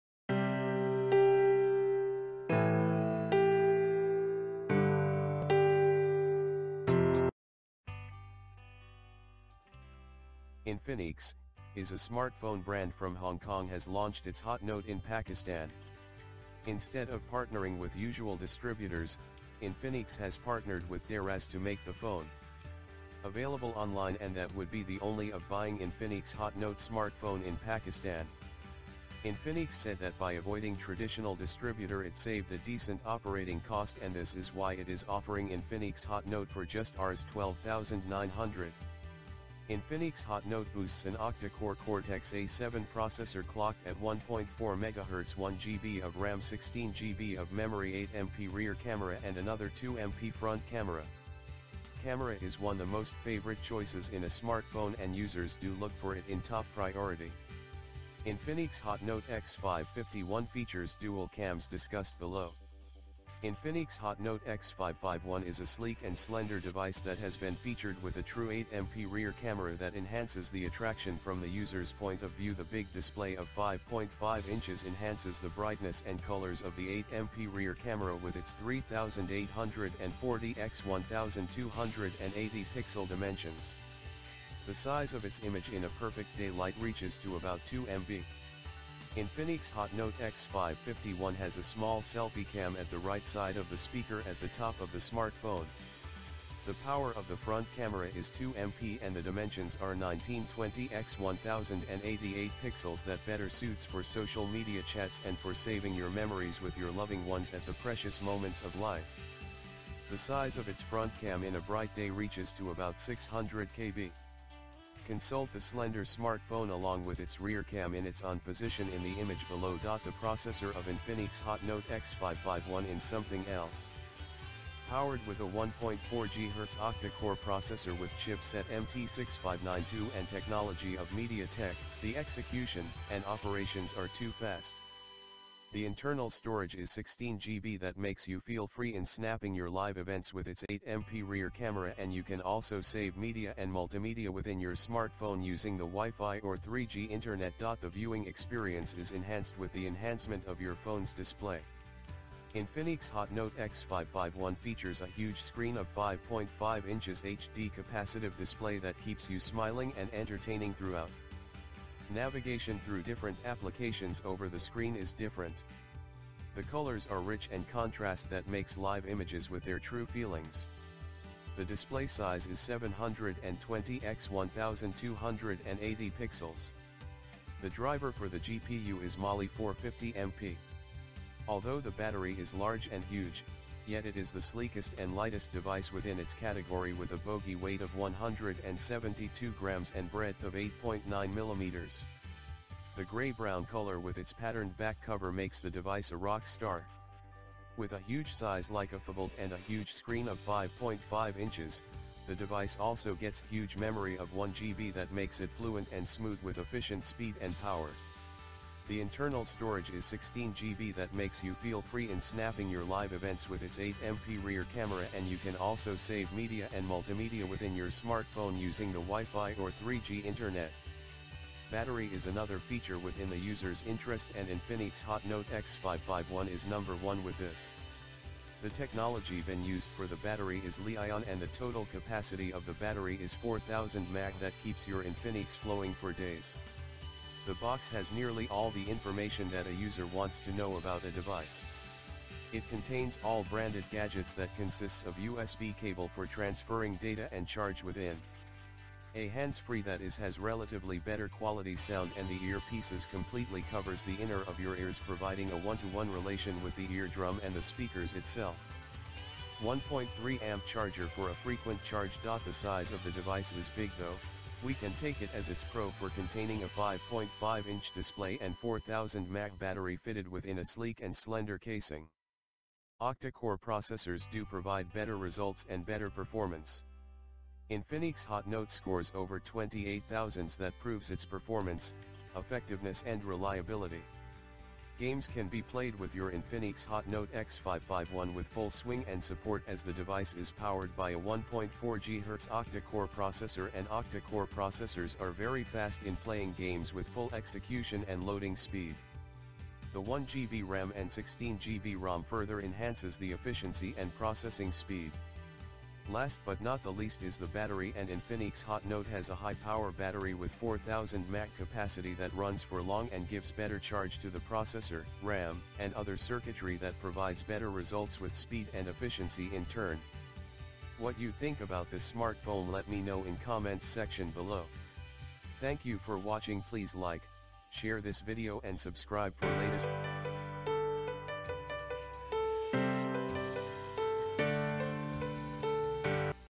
Infinix Hot Note X551 - Review Pakistan
Infinix, is a smartphone brand from Hong Kong has launched its Infinix Hot Note X551 in Pakistan. Instead of partnering with usual distributors, Infinix has partnered with Daraz to make the phone.
Available online and that would be the only of buying Infinix Hot Note smartphone in Pakistan.
Infinix said that by avoiding traditional distributor it saved a decent operating cost and this is why it is offering Infinix Hot Note for just Rs. 12,900. Infinix Hot Note boosts an Octa Core Cortex-A7 processor clocked at 1.4MHz  1 GB of RAM 16 GB of memory 8MP rear camera and another 2MP front camera.
Camera is one the most favorite choices in a smartphone and users do look for it in top priority. Infinix Hot Note-X551 features dual cams discussed below. Infinix Hot Note X551 is a sleek and slender device that has been featured with a true 8MP rear camera that enhances the attraction from the users point of view The big display of 5.5 inches enhances the brightness and colors of the 8 MP rear camera with its 3840 x 1280 pixel dimensions. The size of its image in a perfect day light reaches to about 2 MB.

Infinix Hot Note-X551 has a small selfie cam at the right side of the speaker at the top of the smartphone. The power of the front camera is 2 MP and the dimensions are 1920 x 1088 pixels  that better suits for social media chats and for saving your memories with your loving ones at the precious moments of life. The size of its front cam in a bright day reaches to about 600 KB. Consult the slender smartphone along with its rear cam in its ON position in the image below.

The processor of Infinix Hot Note X551 in something else. Powered with a 1.4 GHz Octa core processor with chip set MT6592 and technology of Media Tek, the execution and operations are too fast. The internal storage is 16 GB that makes you feel free in snapping your live events with its 8 MP rear camera and you can also save media and multimedia within your smartphone using the WiFi or 3G internet.The viewing experience is enhanced with the enhancement of your phones display. Infinix Hot Note X551 features a huge screen of 5.5 inches HD capacitive display that keeps you smiling and entertaining throughout.

Here are unboxing pictures of Infinix Hot Note X551